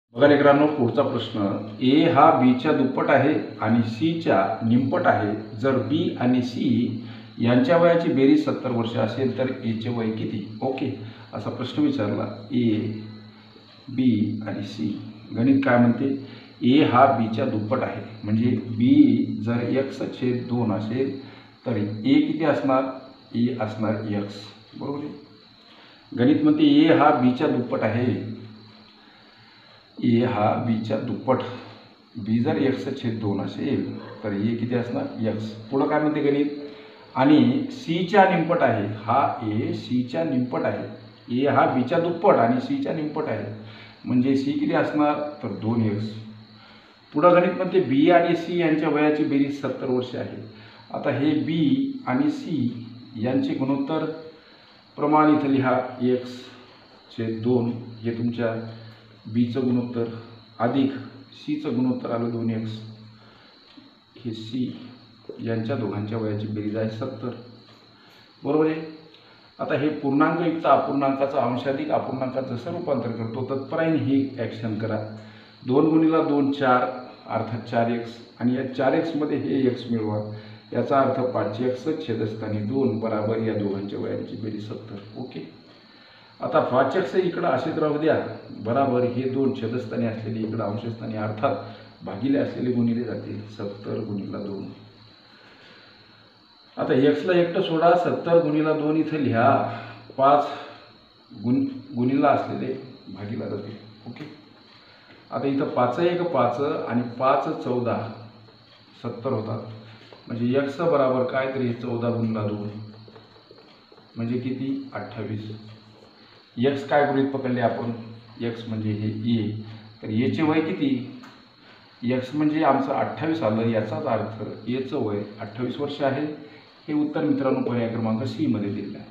A हा B च्या दुप्पट व C च्या निमपट जर B व C यांचे वय 70 असेल तर A चे वय किती?गणितबुद्धीमत्ता#
लेकुराचे हित
स्पर्धा परीक्षा
गणित बुद्धीमत्ता